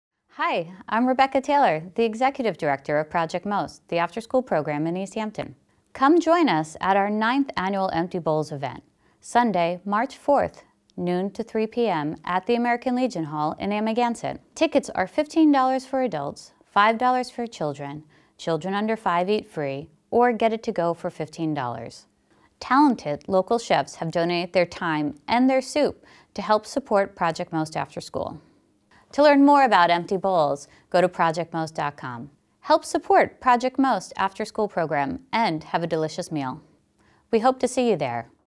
LTV -- Project MOST Empty Bowls 2018 -- PSA
PRODUCER: LTV

This PSA promotes the 9th Annual Empty Bowls Event; proceeds benefit Project MOST, the after school enrichment program at East Hampton and Springs Schools.

Empty Bowls
Sunday, March 4th from 12 - 3PM
Amagansett Legion Hall, Montauk Highway

Produced at the LTV Studio in Wainscott, NY.